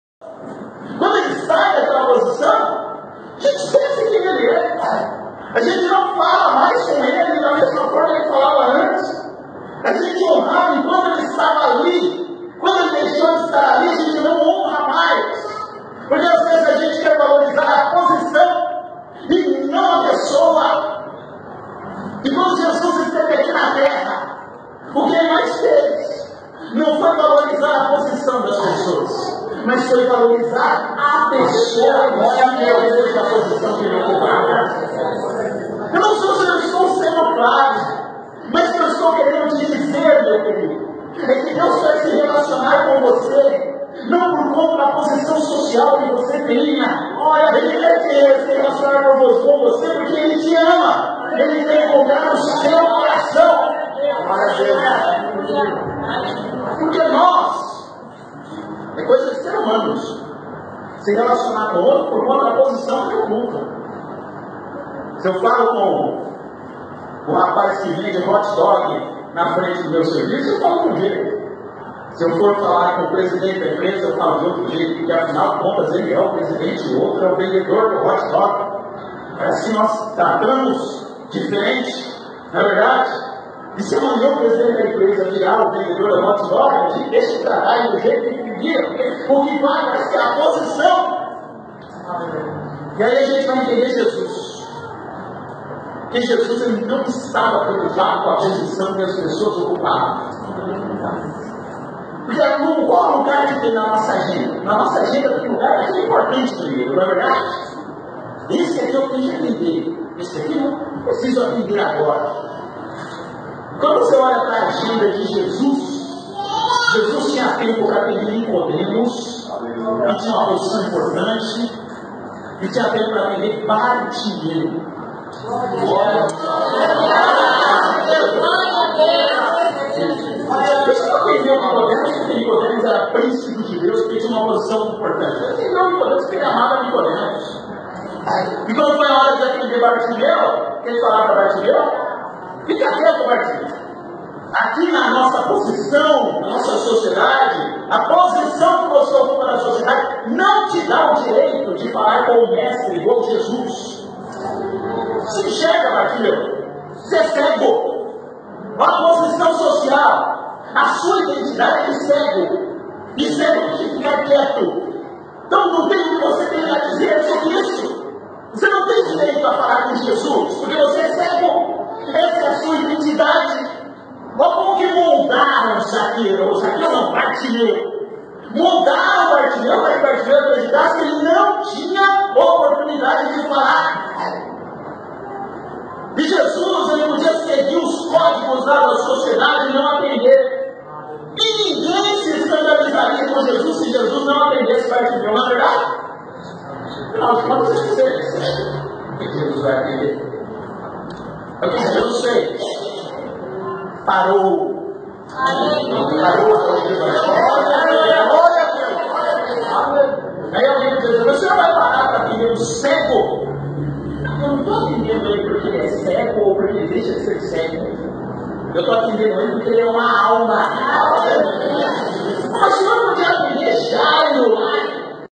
Jesus e as posições sociais - Pr. Maxwell Fajardo
Trecho da mensagem ministrada em 29/04/2018 na AD Santo André - Juta II na Escola Bíblica para Jovens - Pr. Maxwell Fajardo